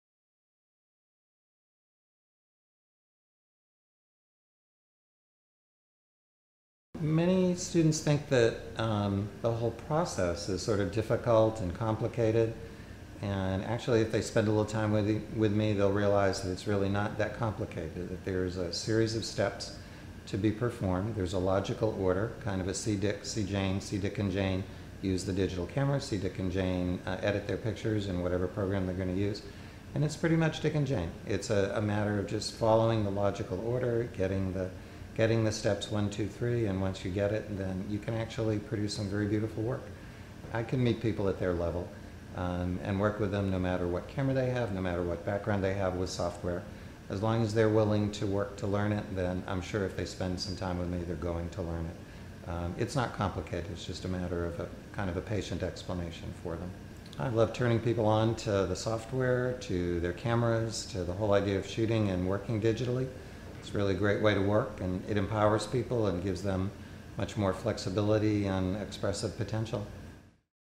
Eliot Cohen - Photoworks Faculty Member
Eliot Cohen is one of the area's best known digital educators, reknowned for his ability to explain digital procedures in a clear, logical manner. Eliot teaches workshops nationally and internationally, and consults with individuals wishing to improve their skills in digital photography and editing.